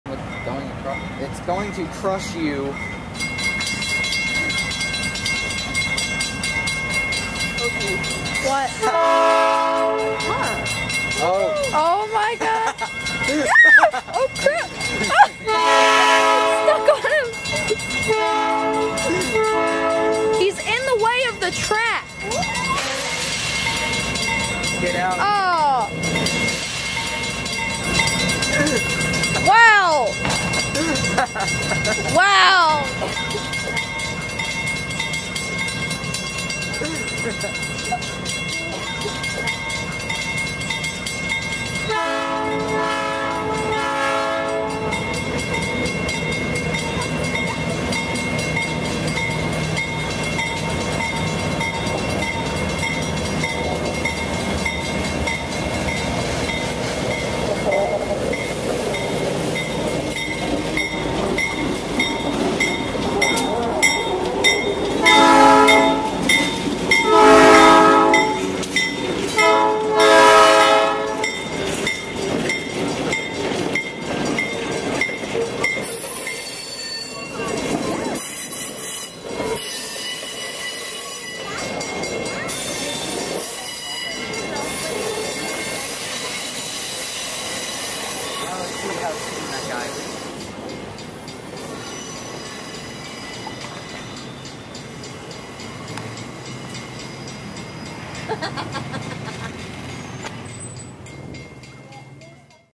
Train crossing gate comes down on pickup truck in New Hope PA
A truck gets caught under the crossing gate at New Hope railroad